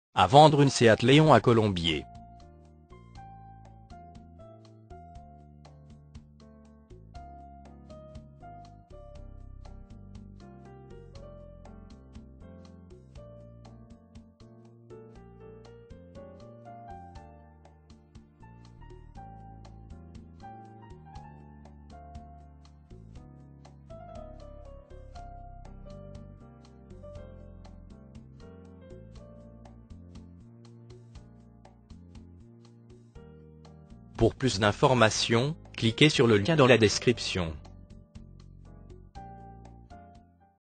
Achat Vente une Seat Leon Colombiers
A vendre à  Colombiers  Occasion automobile Seat Leon de 95000 Km.
automobile occasion Autoenvideo : Achat Vente une Seat Leon  Colombiers  Hérault

Création du diaporama vidéo par API pour professionnel de l automobile